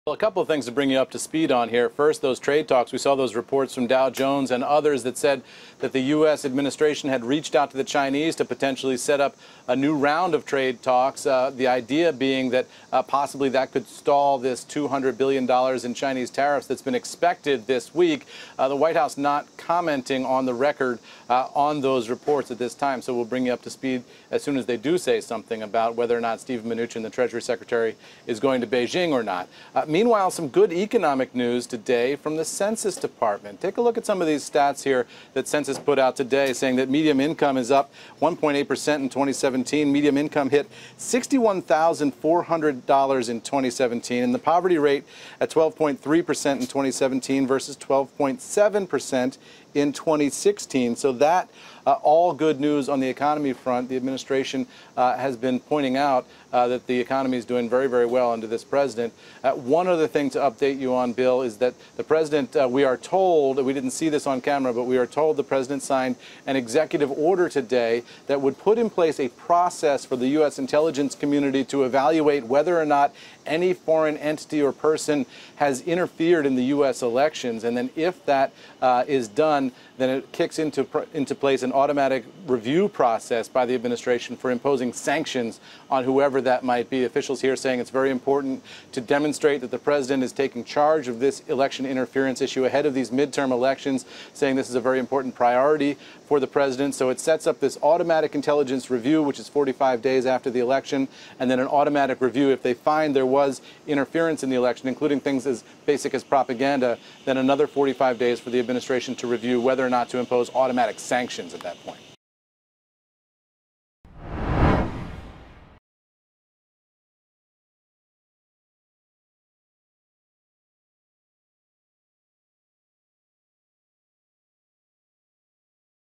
US proposes a new round of China trade talks, US household incomes rise
CNBC's Eamon Javers reports that the U.S. has proposed a new round of trade talks with China and that the Census Bureau reported a rise in U.S. household incomes. Javers also reported that President Donald Trump signed an executive order allowing sanctions on foreign entities if they meddle in U.S. elections.